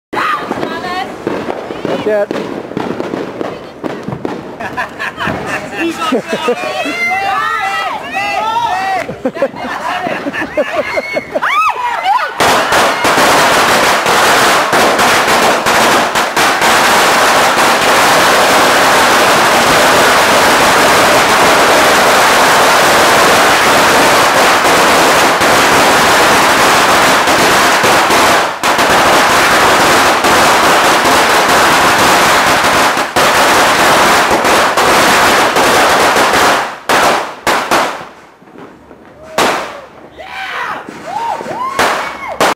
New Years Eve Lightin Judas Belt drunk in the Philippines
New Years Eve in the Philippines
fireworks + alcohol = funny
Judas belt is 2000 rounds